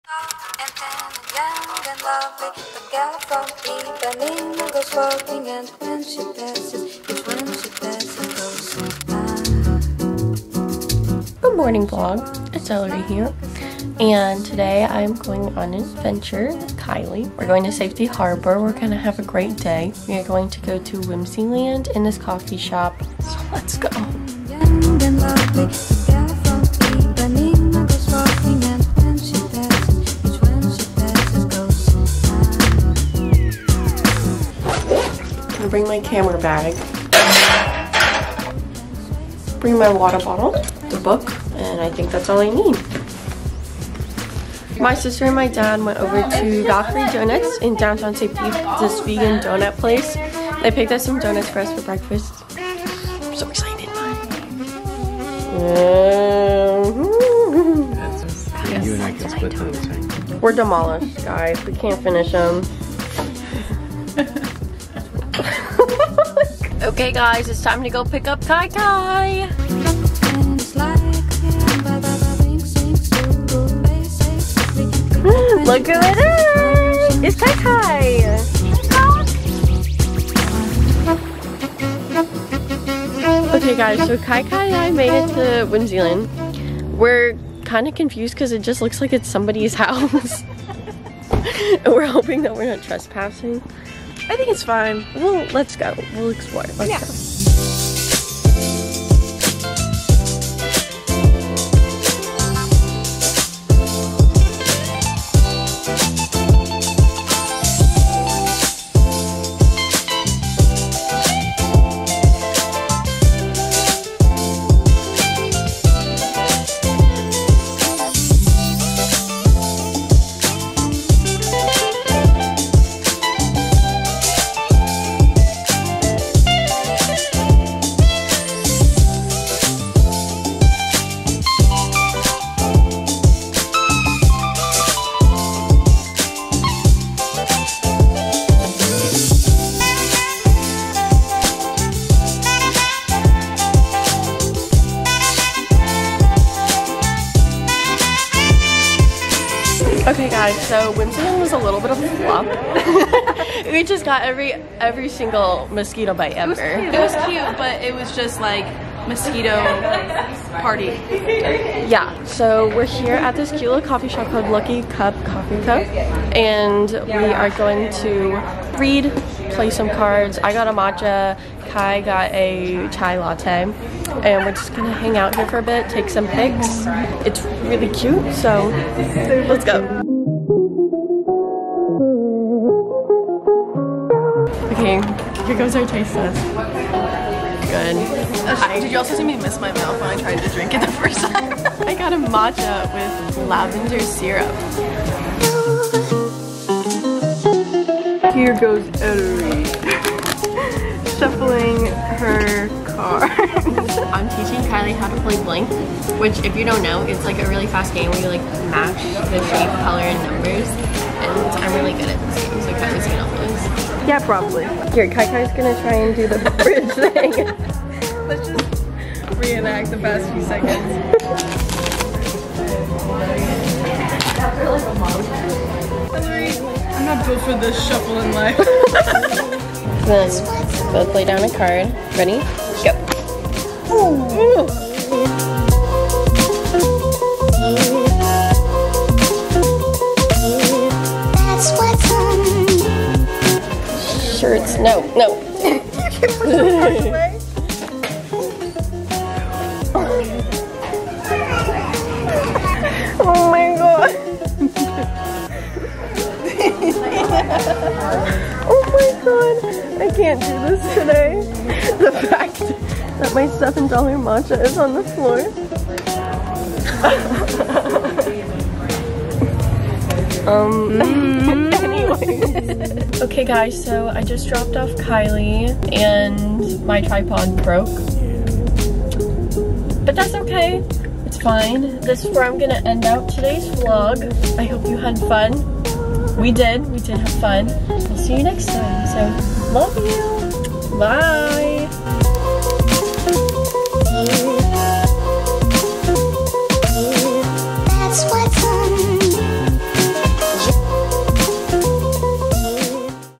a trendy day in safety harbor!! // safety harbor florida vlog august 2021
hey u!! have a fantastic day!!

hey guys!! in this video kyky and i venture to safety harbor and go to whimseyland and a cute coffee shop!! hope u guys enjoy :)

can we get a rip in the chat for my matcha pls.

about me:
age: 17
edited on: final cut pro x
filmed on: canon eos m50